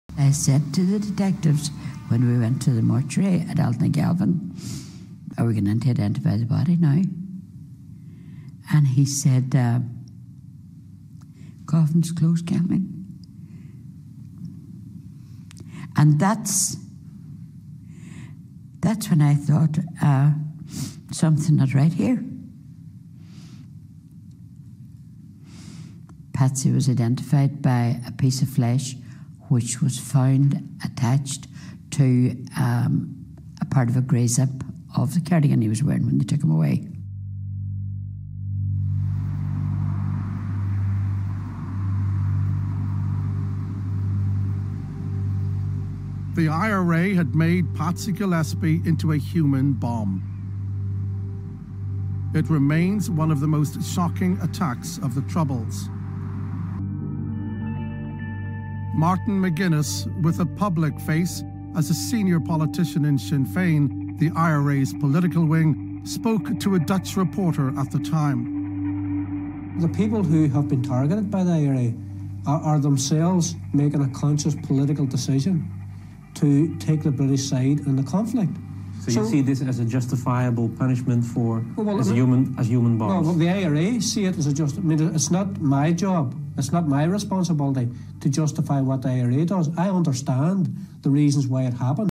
IRA Human Bomb victim's wife recounts what happened to her husband Patsy Gillespie in 1990.
Kathleen Gillespie recounts what happened to her husband when the IRA made him into a human bomb.  They strapped him into a van containing 1200lb of explosives.

"Patsy was identified by a piece of flesh which was found attached to a part of a grey zip of a cardigan he was wearing when they took him away"

Martin McGuinness, Sinn Fein Deputy First Minister and former IRA commander justifies Patsy's death.